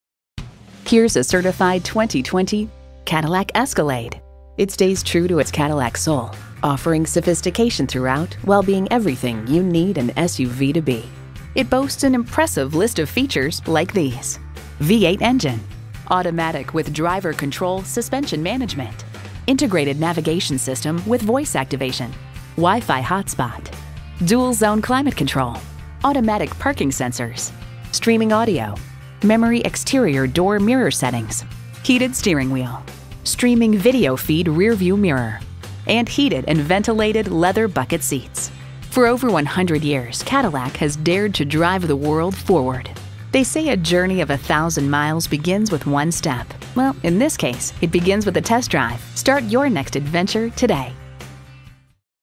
Certified 2020 Cadillac Escalade Tampa FL St. Petersburg, FL TR11893 - SOLD
Year: 2020
Make: Cadillac
Model: Escalade
Trim: Luxury
Engine: 6.2l 8 Cylinder Fuel Injected
Transmission: RWD Automatic
Color: Black Raven
Interior: Black
Mileage: 21022
Stock #: TR11893
VIN: 1GYS3BKJ1LR144457

Address:
101 East Fletcher Avenue